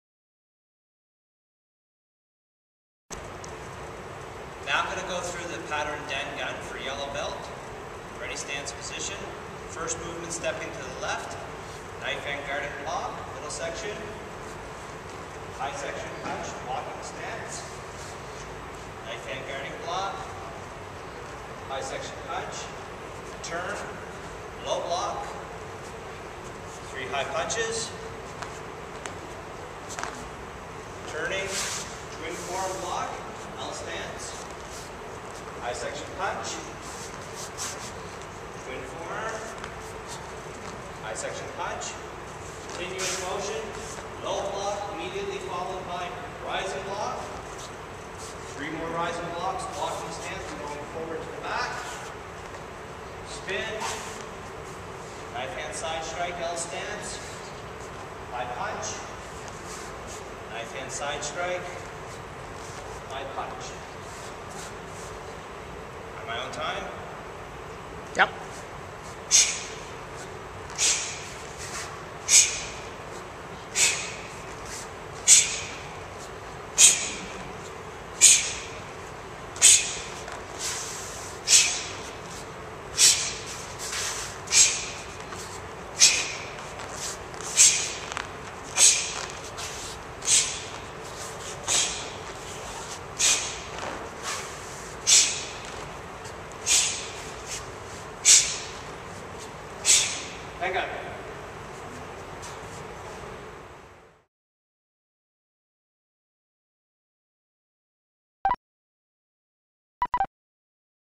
Dan-Gun
DAN-GUN is named after the holy Dan-Gun, the legendary founder of Korea in the year of 2,333 B.C.

For more information about any of our programs, please visit us at www. carrtaekwondo.com